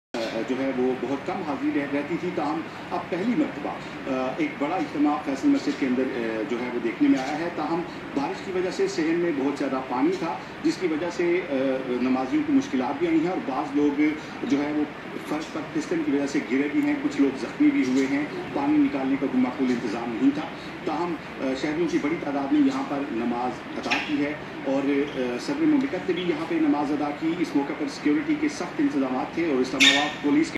Federal Cabinet perform Eid Namaz.///Faisal masjid me naya inqelab dekhne ko mil rha hai.short#
Federal cabinet performing eid namaz in Faisal Masjid but doing 😮😮😮😮short video